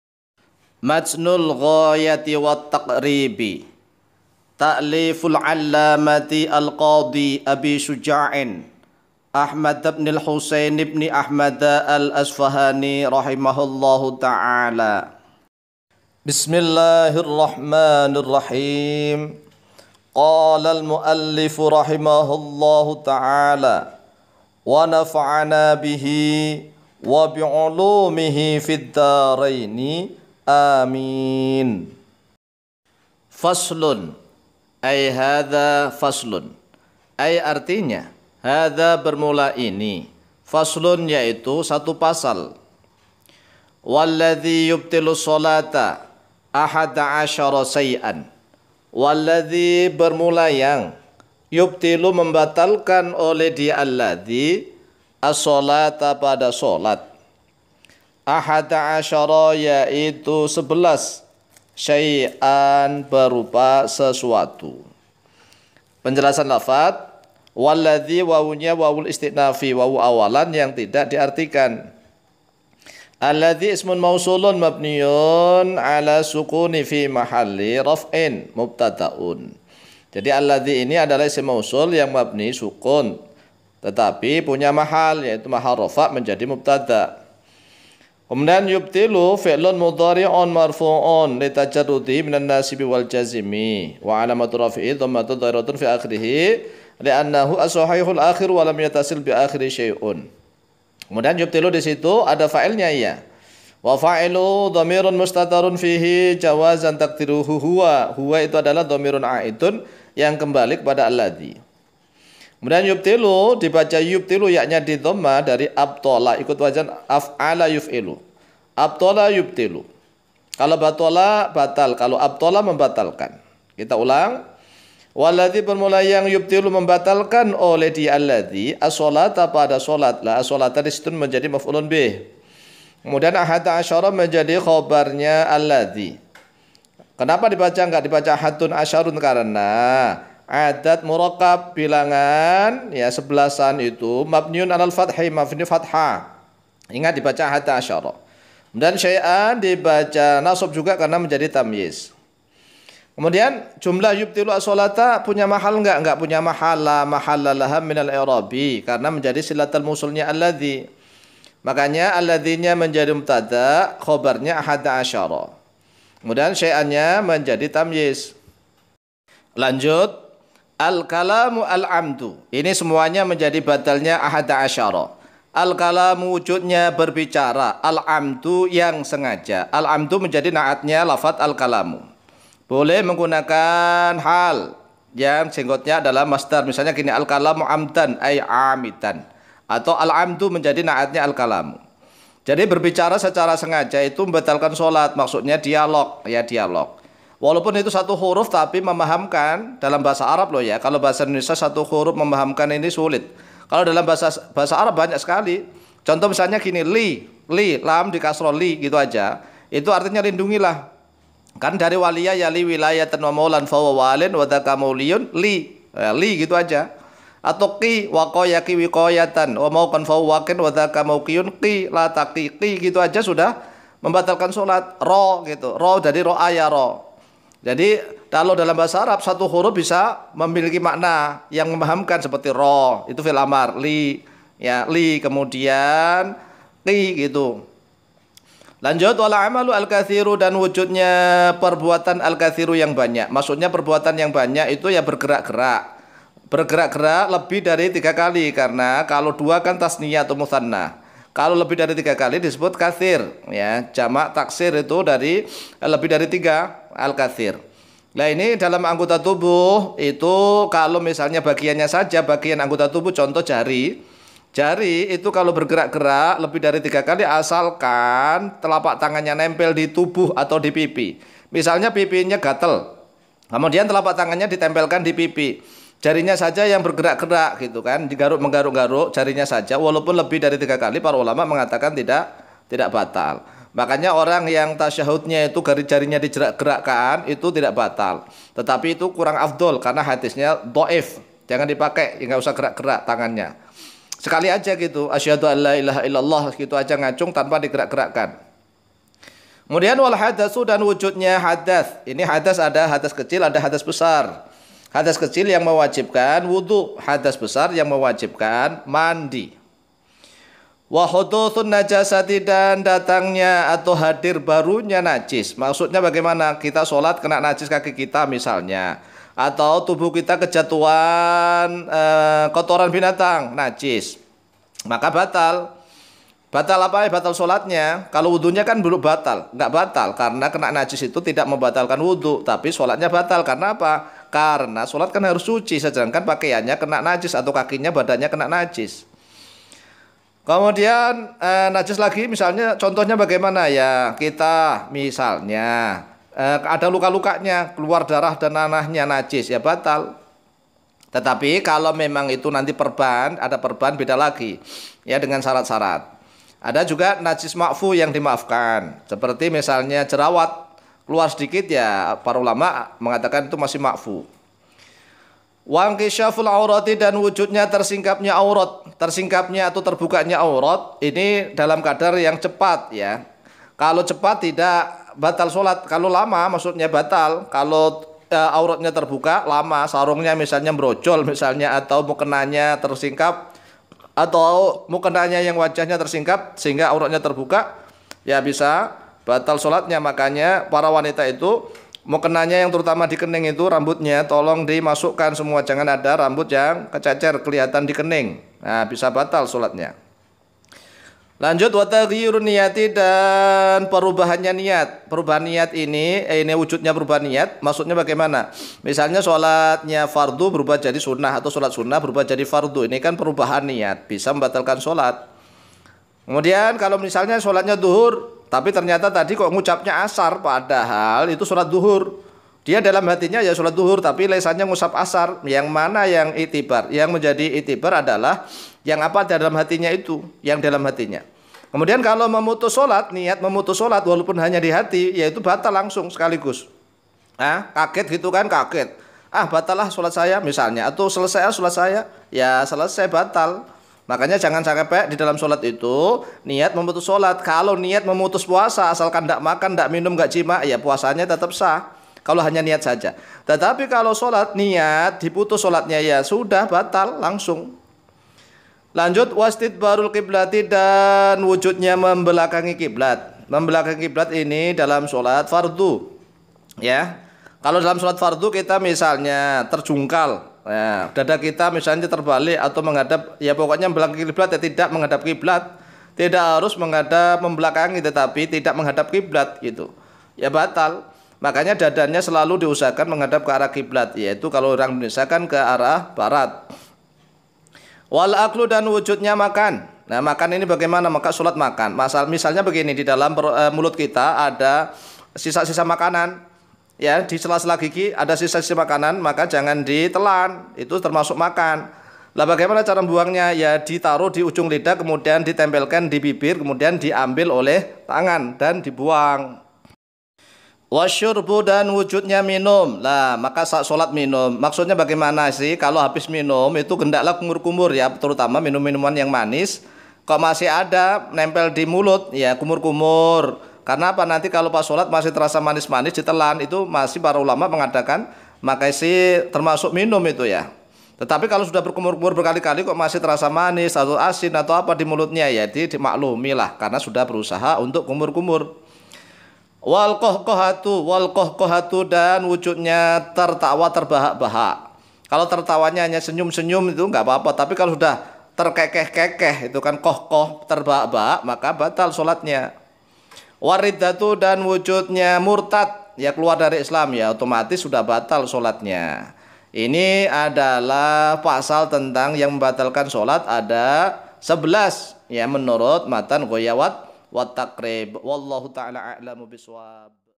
Eps.23 والذي يبطل الصلاة YANG MEMBATALKAN SHOLAT
(فصل) والذي يبطل الصلاة أحد عشر شيئا: الكلام العمد، والعمل الكثير، والحدث، وحدوث النجاسة، وانكشاف العورة، وتغيير النية، واستدبار القبلة، والأكل، والشرب، والقهقهة، والردة.